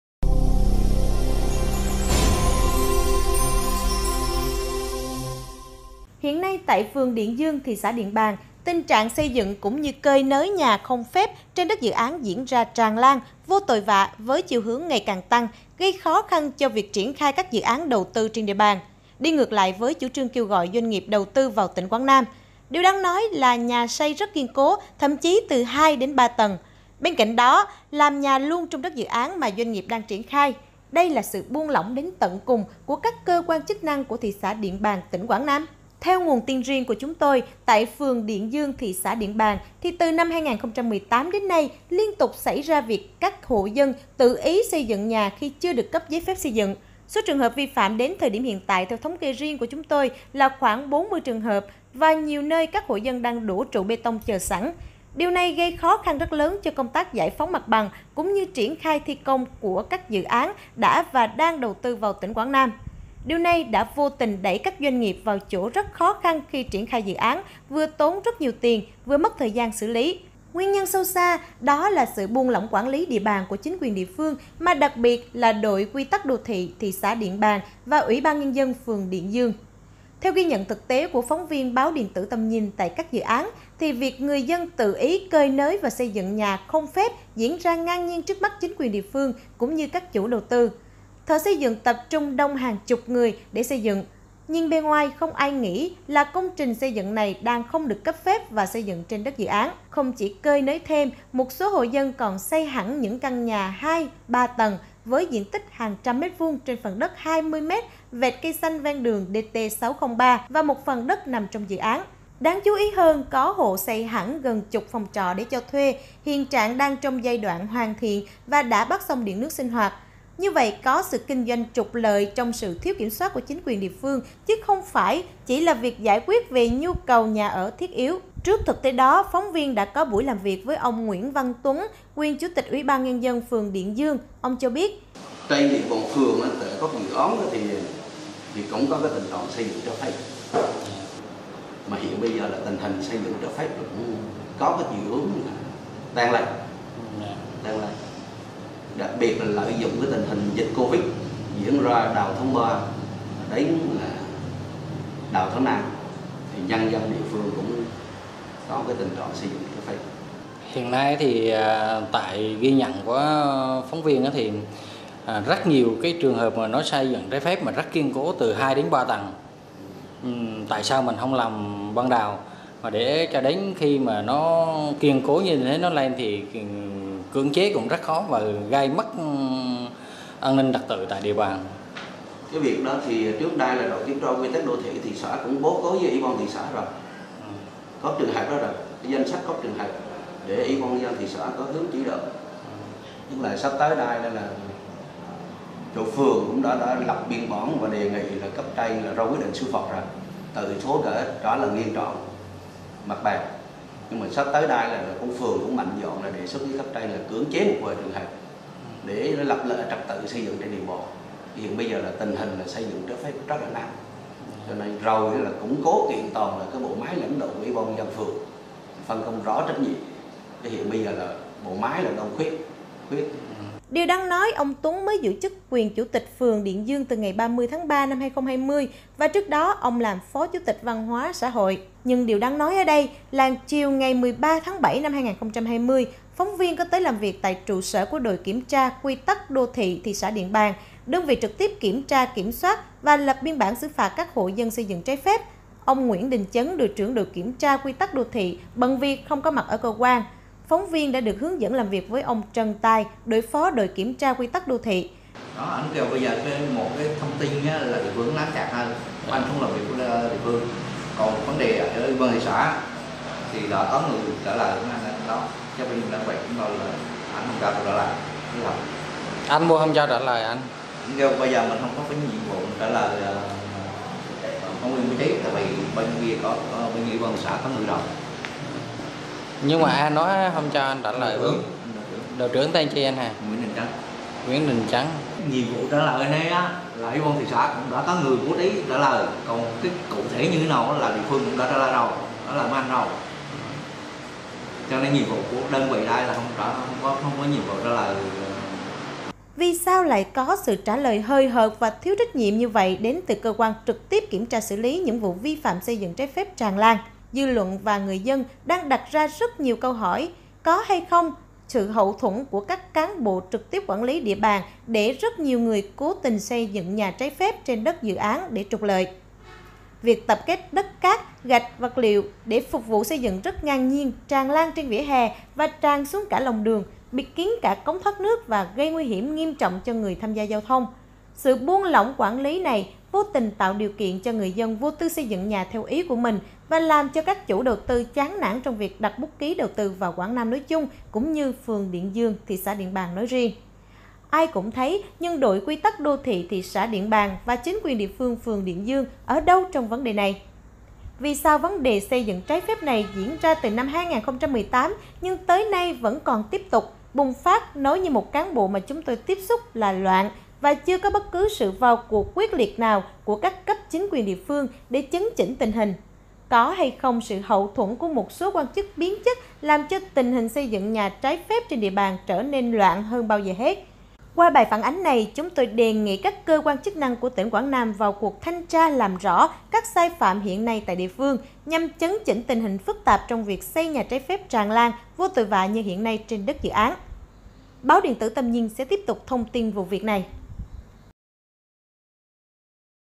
Quảng Nam: Tình trạng xây dựng nhà trái phép tràn lan trên đất dự án?
Tầm nhìn TV: Hiện nay tại phường Điện Dương – Tx Điện Bàn, tình trạng xây dựng cũng như cơi nới nhà không phép trên đất dự án diễn ra tràn lan, vô tội vạ với chiều hướng ngày càng tăng, gây khó khăn cho việc triển khai các dự án đầu tư trên địa bàn. Đi ngược lại với chủ trương kêu gọi doanh nghiệp đầu tư vào tỉnh Quảng Nam. Điều đáng nói là nhà xây rất kiên cố, thậm chí từ 2 đến 3 tầng. Bên cạnh đó, làm nhà luôn trong đất dự án mà doanh nghiệp đang triển khai. Đây là sự buông lỏng đến tận cùng của các cơ quan chức năng của Thị xã Điện Bàn tỉnh Quảng Nam?